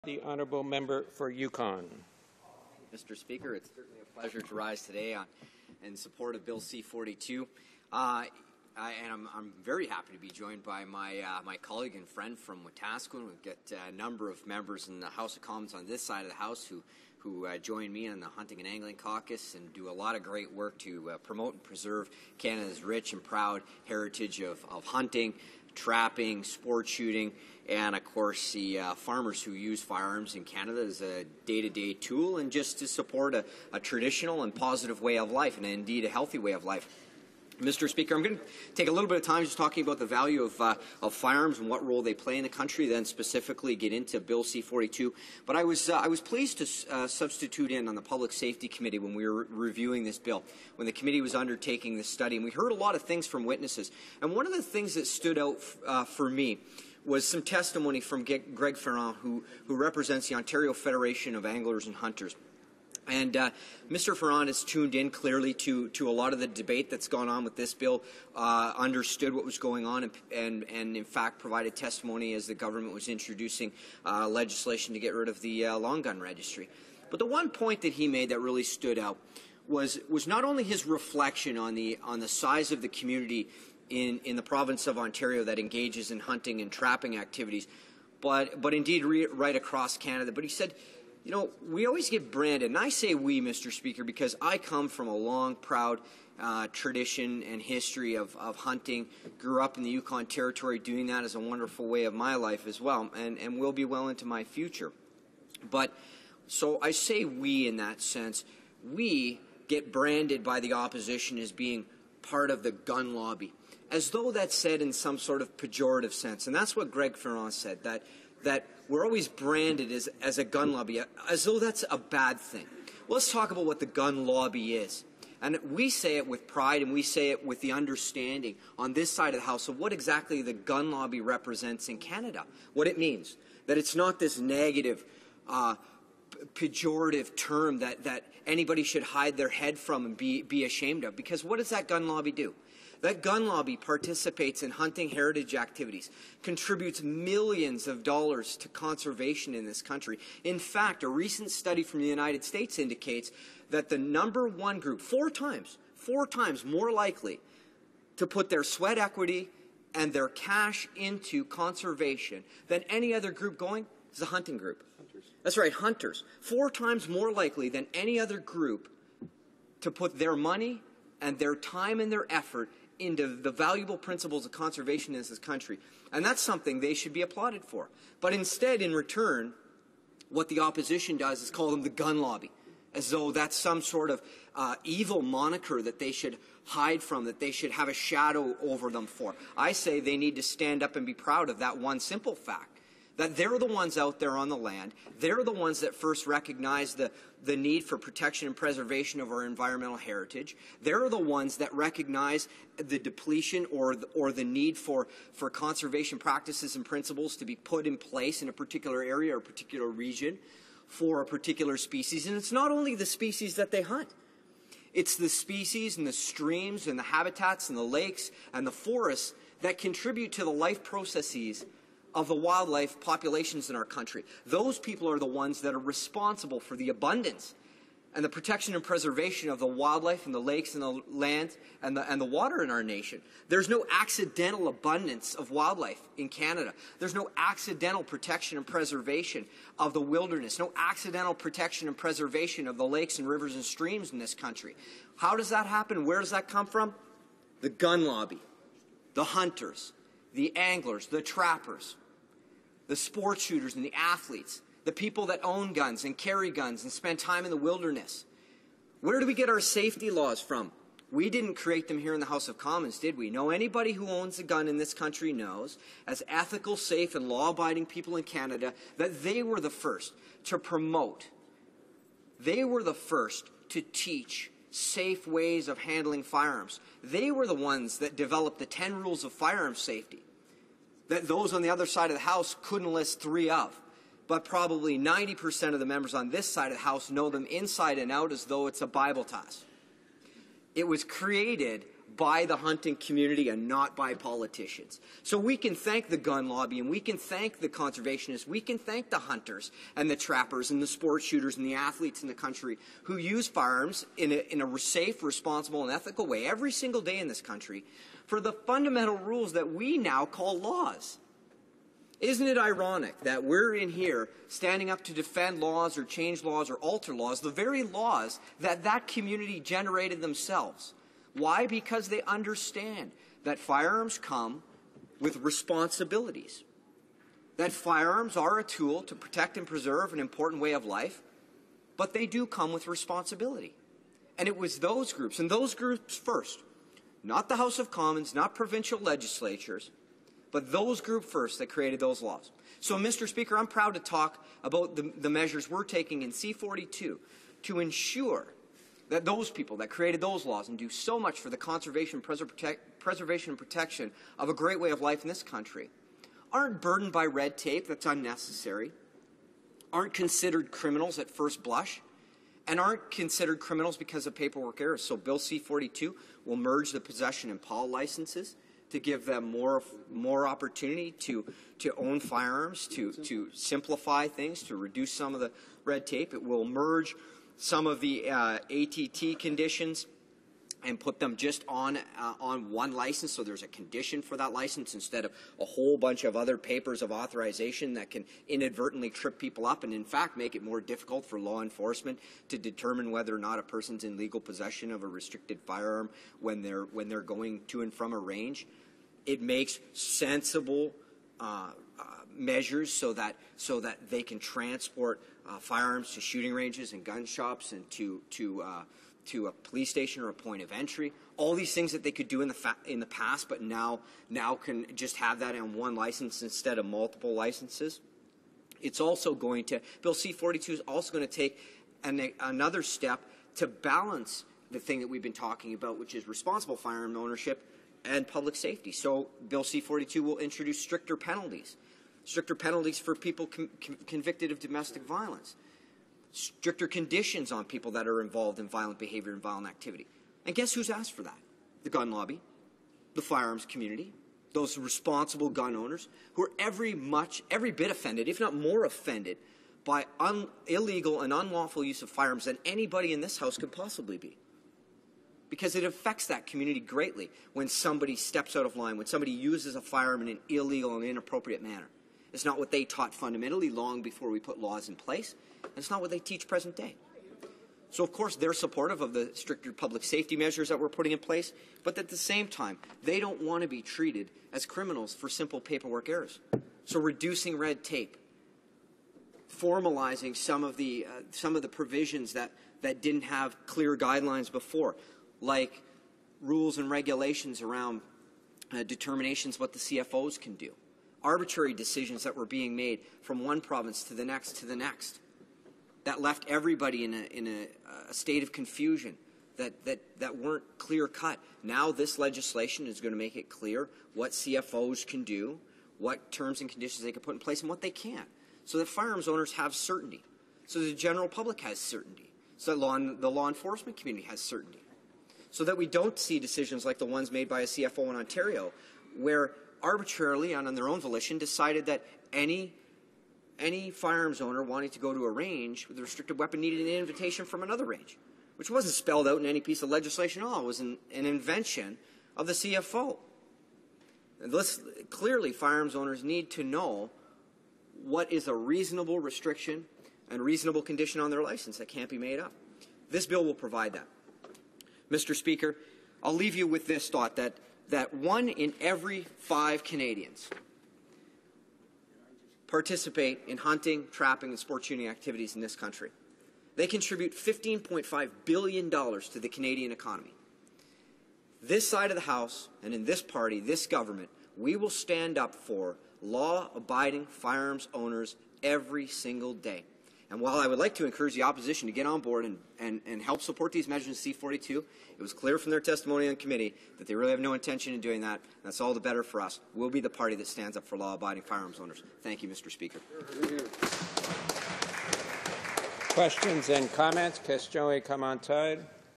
Who is Canada's Gun Lobby?
Ryan Leef, former MP for the Yukon
House of Commons, March 15 2015